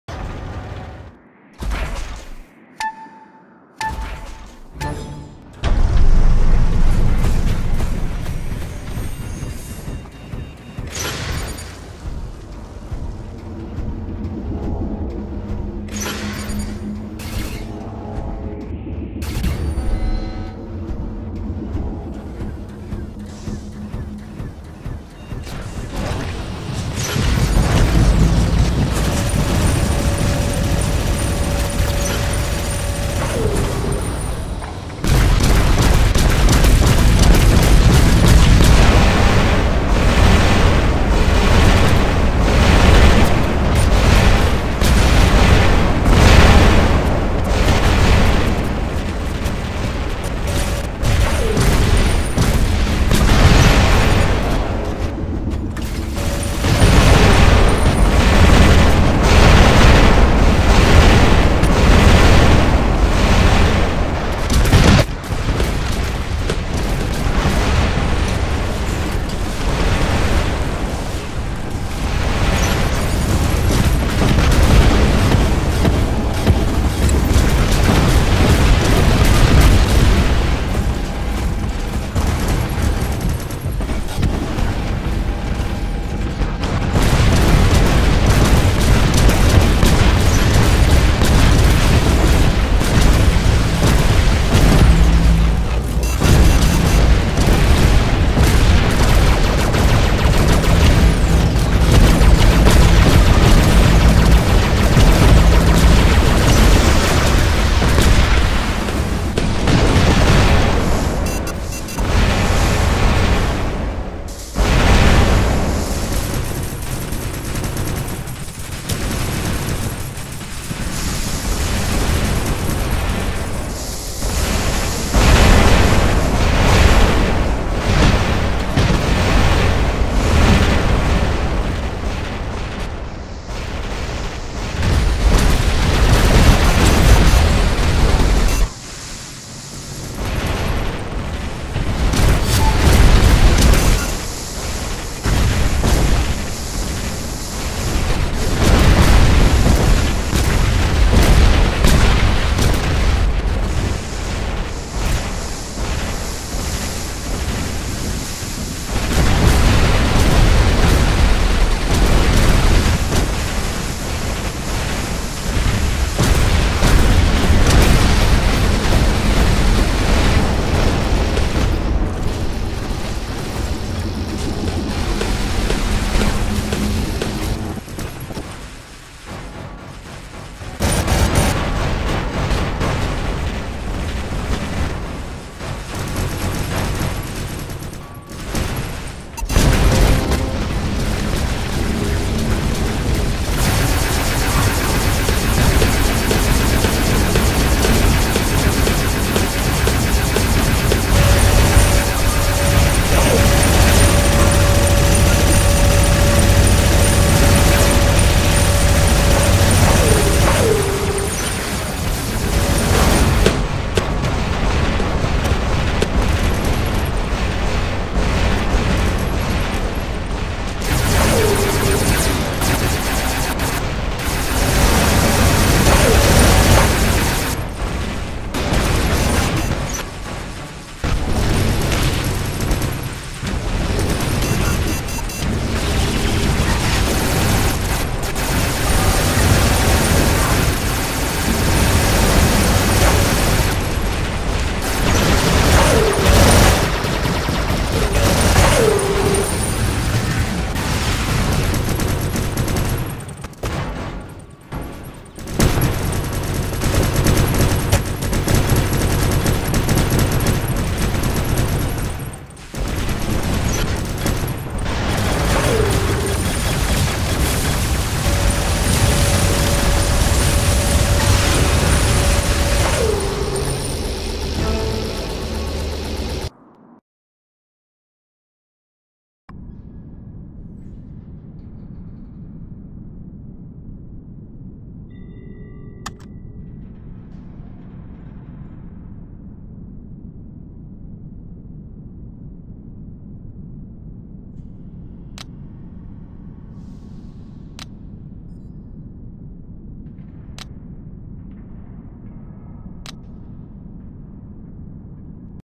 This is how BR is played
Lucked up on a good team. Got the most beacons with 0 kills, but a win is a win.